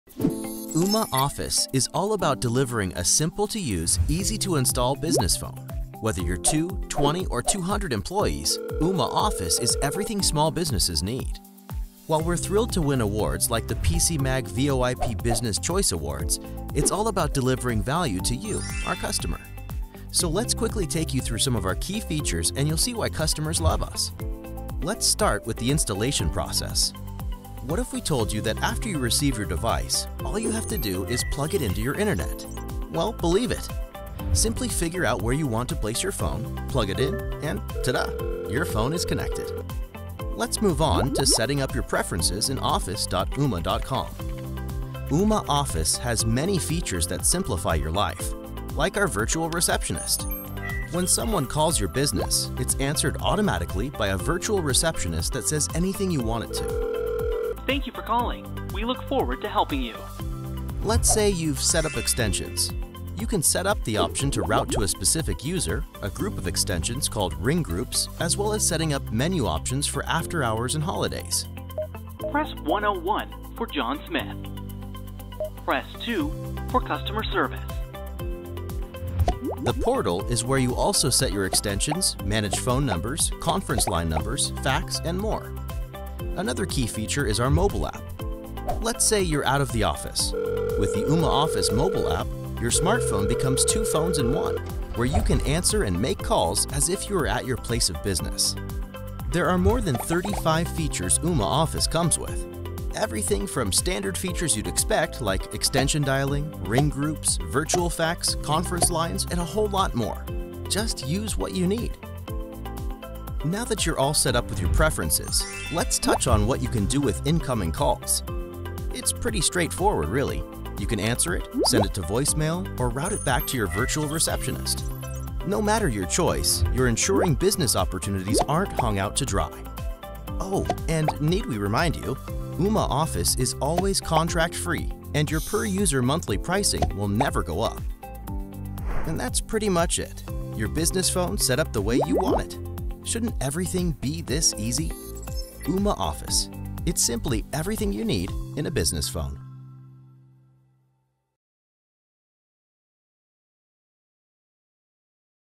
Ooma Office Business Phone Overview
Whether you have 2, 20, or 200 employees, Ooma Office has everything you need in a business phone. Named as the Best Business VoIP Service by PC Magazine, Ooma Office business phones are easy to install and have more than 25 great features like a virtual receptionist, conference lines, and a smart phone app that allows you to take your office calls anywhere right on your mobile phone. to learn more.